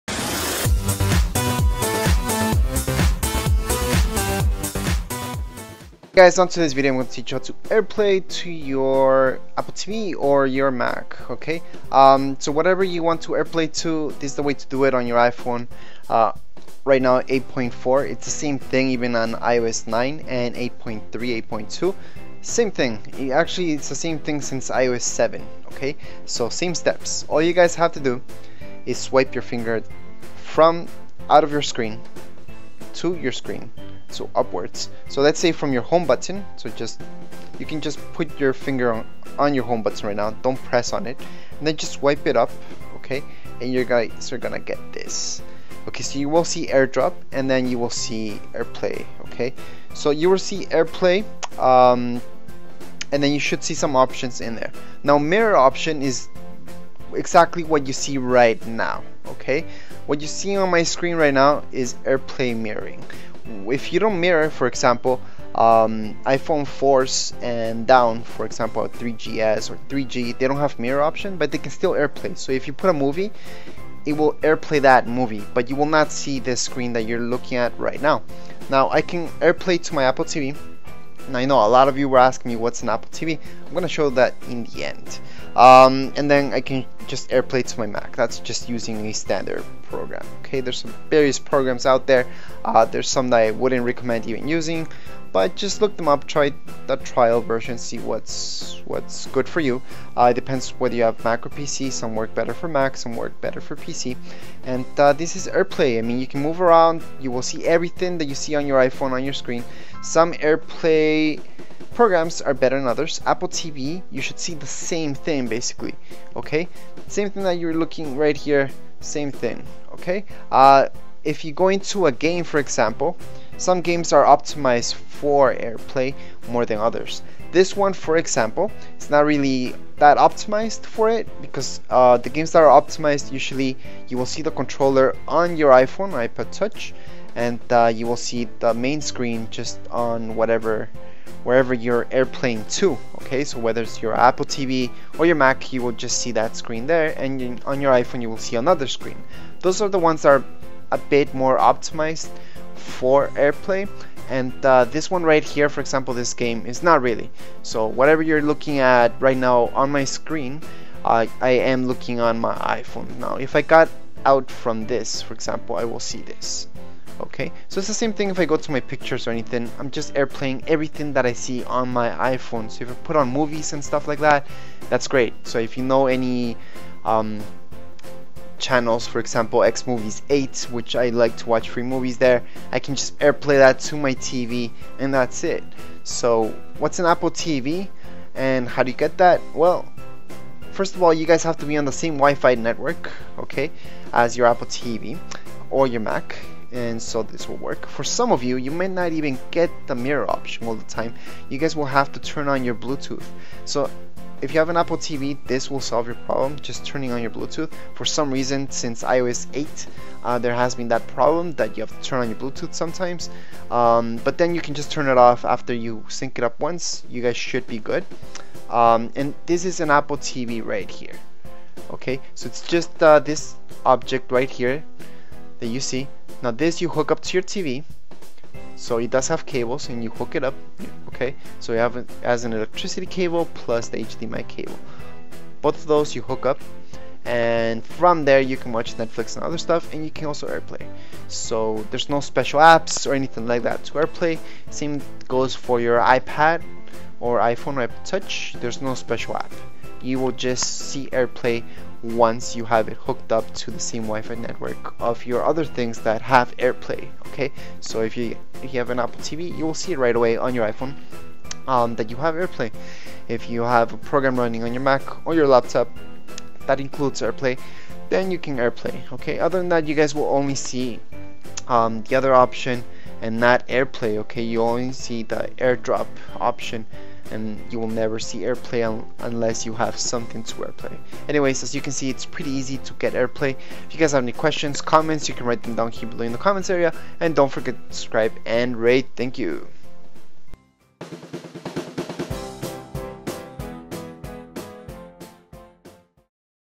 Airplay in iOS 8.4 How to airplay iPhone iPad iPod touch
Airplay manual
2015